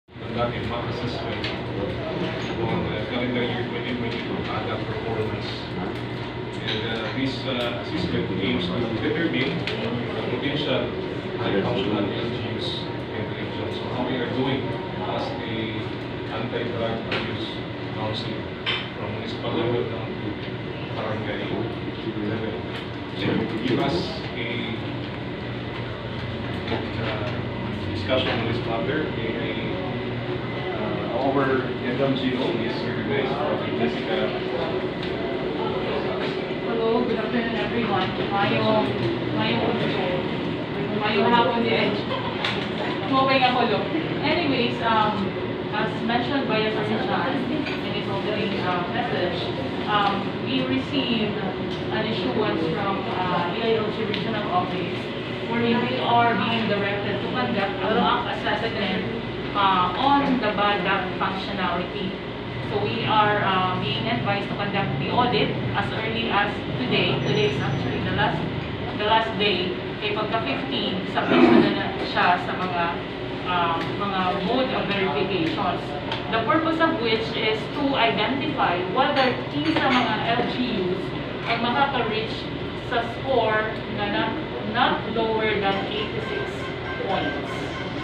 MADAC SPECIAL MEETING @ Hilongos, Leyte
HILONGOS, LEYTE - Malampusong nahimo ang gipatawag nga Special Meeting sa Municipal Anti-Drug Abuse Council (MADAC) kagahapon, adlawng Lunes, sa alas 2 ang takna sa hapon didto sa Mayor's Conference Room, 2nd Flr., Municipal Hall ning lungsod.

Kini gihimo gumikan sa Memorandum Circular nga gipagawas sa Department of Interior and Local Government (DILG), kaniadtong ika-21 sa Pebrero, ning tuiga kabahin sa Mock Assessment CY-2022 Anti-Drug Abuse Council (ADAC) Performance Audit.

Ang naasoyng meeting, gipangunahan ni Municipal Mayor Manuel R. Villahermosa uban ni Municipal Administrator - Atty. Lemuel Jan Nerves, SB-Secretary Catherine Fabular, PMaj. Demain, DILG, Municipal Health Unit, Dep-Ed, Civil Society Organization sama sa CARE Media Observer nga gitambungan ni Executive Officer Mr. Eric F. Justiniane ug uban pa.